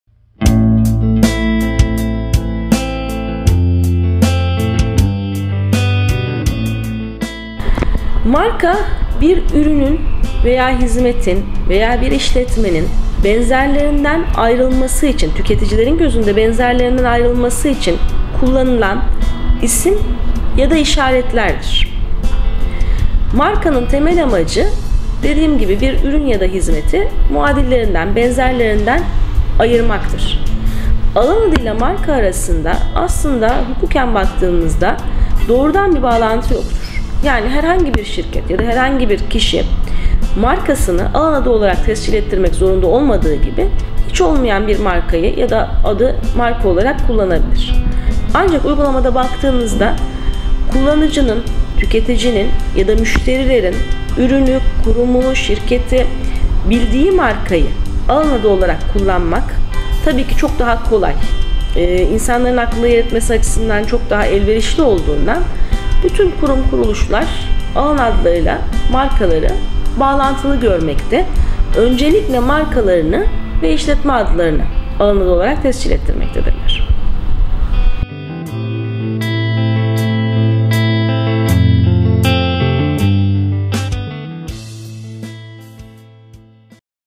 AV. Ceyda Cimilli AKAYDIN [HukukTV.com]
Marka nedir? Alan adı ve marka arasındaki bağlantı nedir?

Marka, bir işletmenin mal ve/veya hizmetlerini bir başka işletmenin mal ve/veya hizmetlerinden ayırt etmeyi sağlaması koşuluyla, kişi adları dahil, özellikle sözcükler, şekiller, harfler, sayılar, malların biçimi veya ambalajları gibi çizimle görüntülenebilen veya benzer biçimde ifade edilebilen, baskı yoluyla yayımlanabilen ve çoğaltılabilen her türlü işarettir.